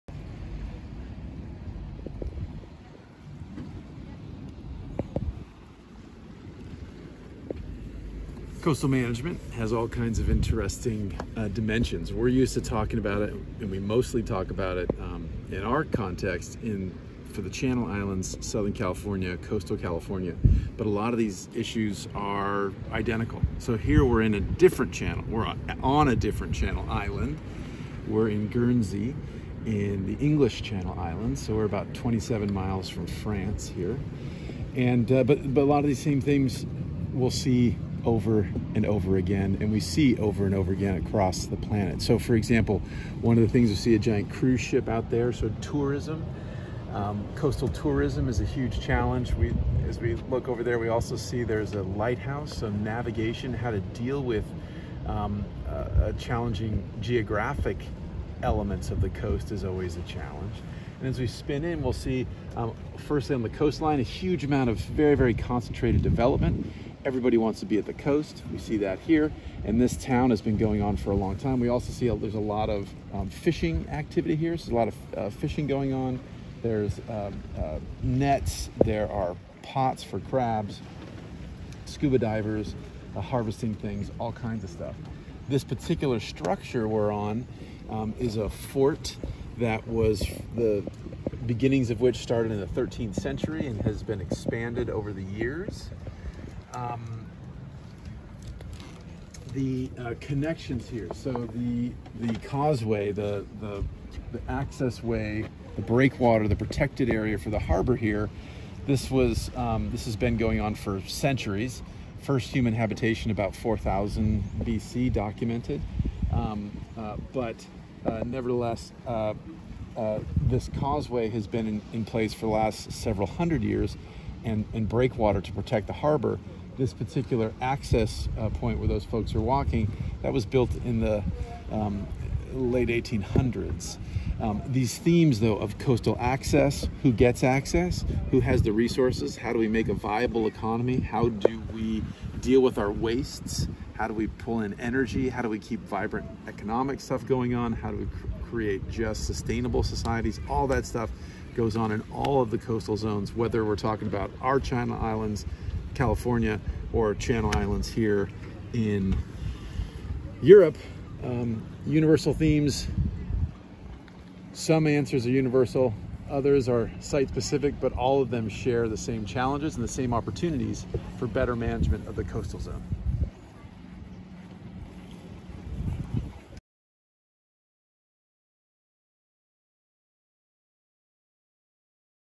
Coastal Management from a DIFFERENT Channel Island
This was filmed from Castle Cornet at the entrance to Saint Peter Port on the on the Bailiwick of Guernsey. Guernsey is  the largest island of the network of islands off France's Normandy coast that are collectively known and the (English) Channel Islands. These English-speaking islands have a long history or extracting and protecting resources across the coastal zone.